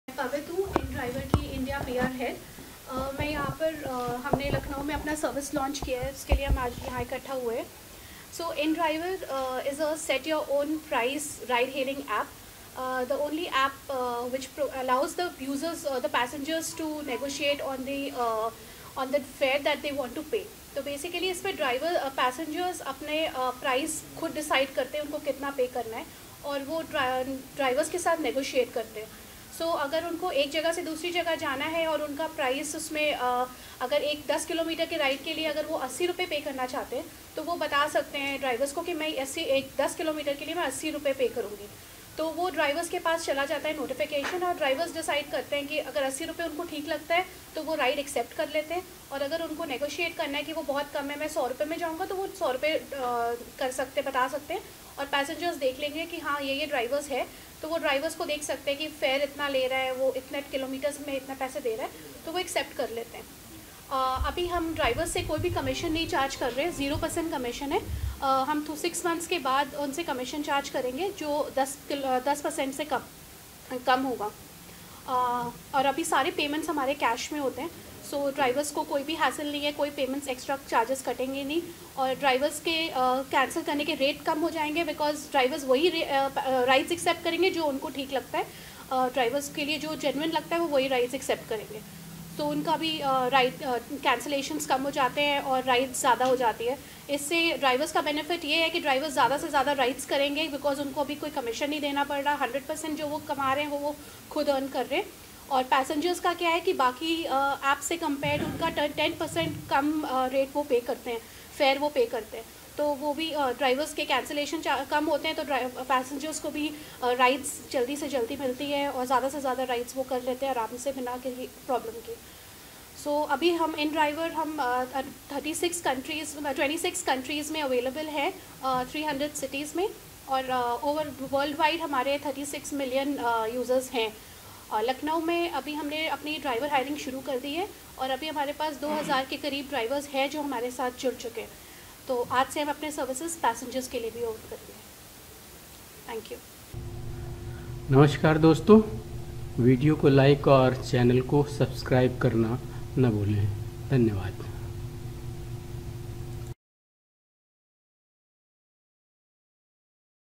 inDriver Set-Your-Own-Price Ride-Hailing App launched in Lucknow
inDriver ने कैब यूजर को दी मोलभाव की शक्ति